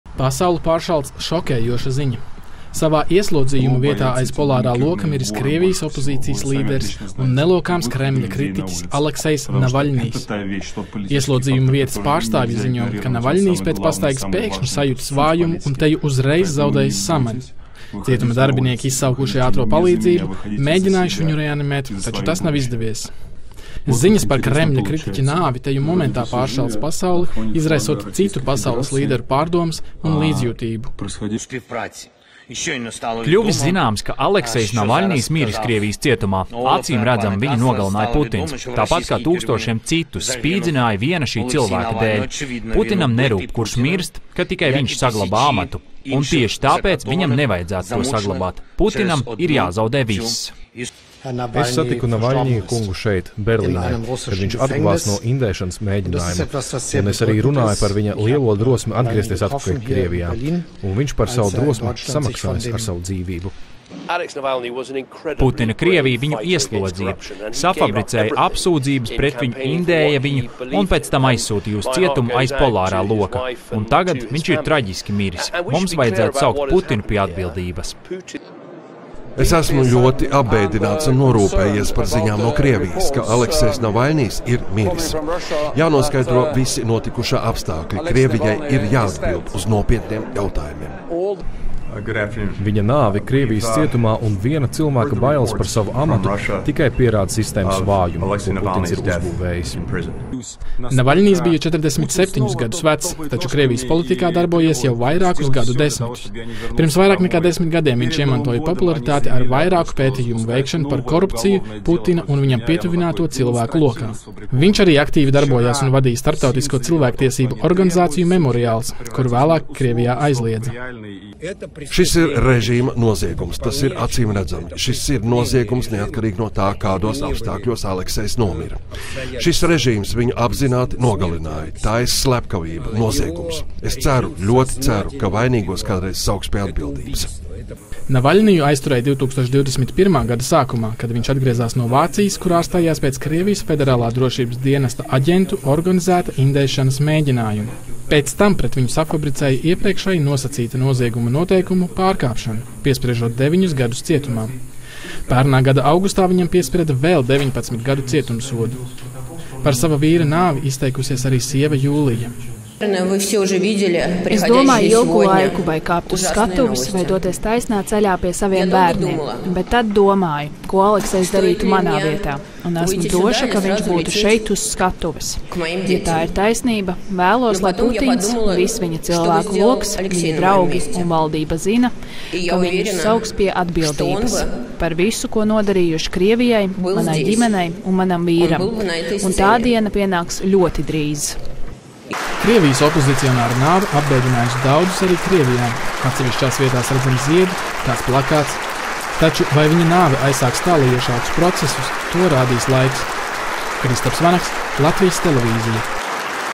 Aleksejs Navaļnijs miris savā ieslodzījuma vietā
Krievijā savā ieslodzījuma vietā miris Krievijas opozicionārs un Kremļa kritiķis Aleksejs Navaļnijs. Viņš bija opozīcijas redzamākā seja un lielākais Putina pretinieks. Navaļnija nāves apstākļi joprojām tiek noskaidroti, taču daudzi pasaules līderi izteikuši līdzjūtību, kā arī dusmas par opozicionāra nāves apstākļiem.